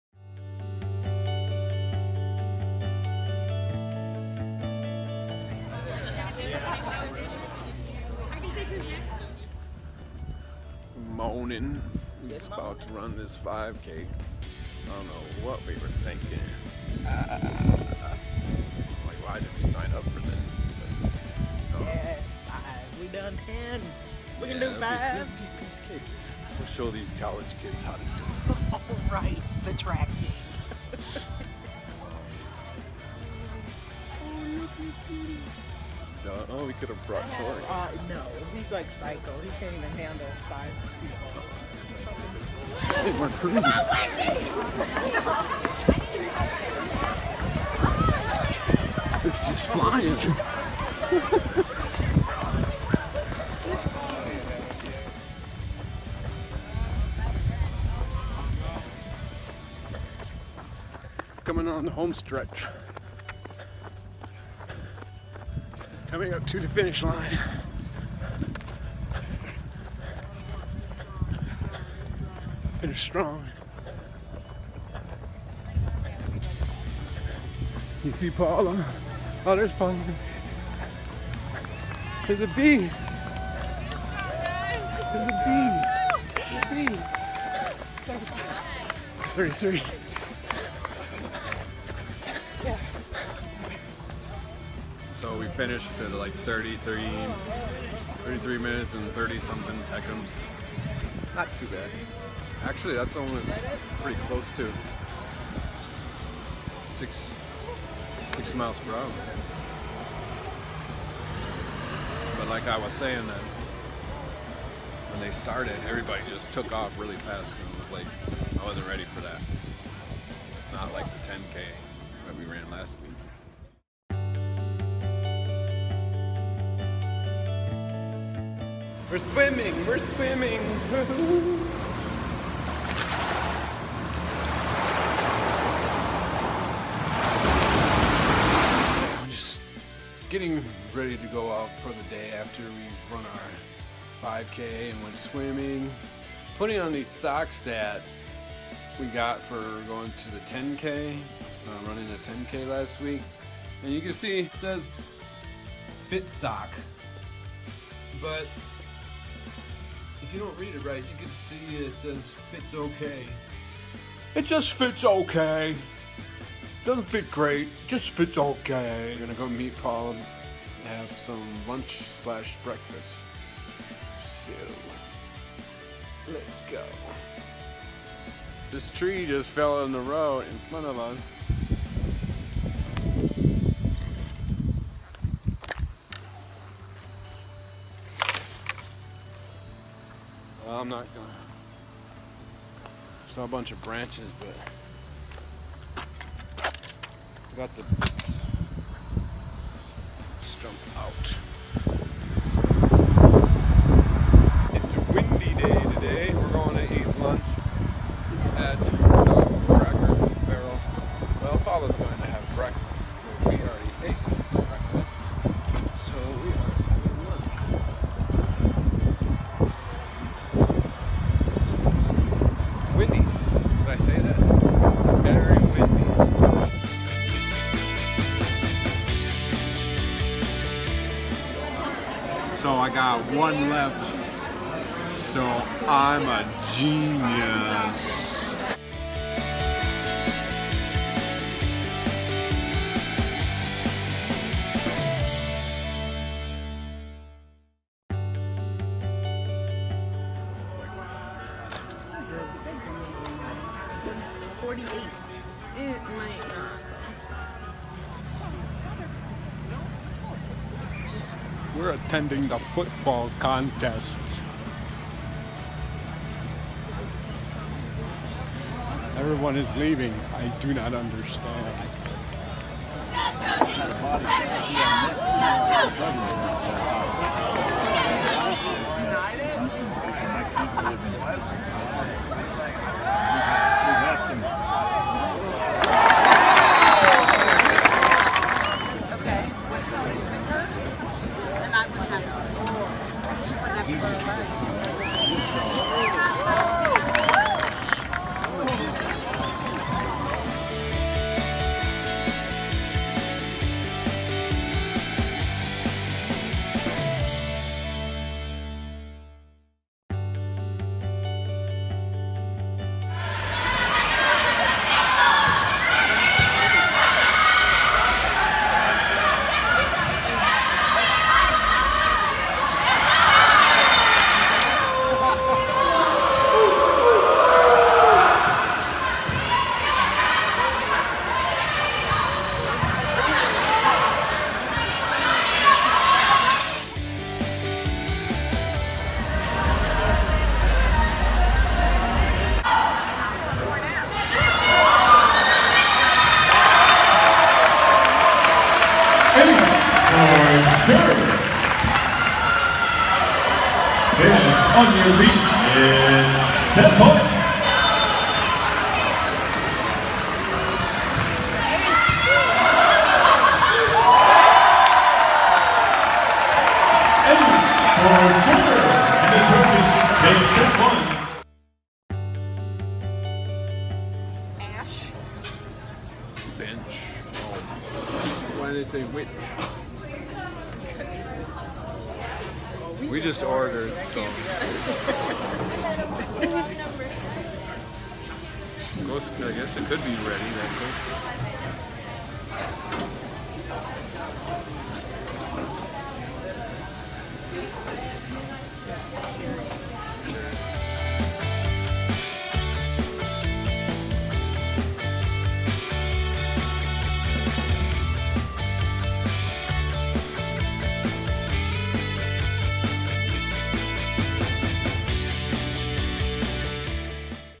Paulita & Scott Vlogs Fall Parent Council 3
We go back to Taylor University for Homecoming and to serve on the Parent Council.  In this video we run a 5K, and go to the Football game and Volleyball game.  Part 3 of 4.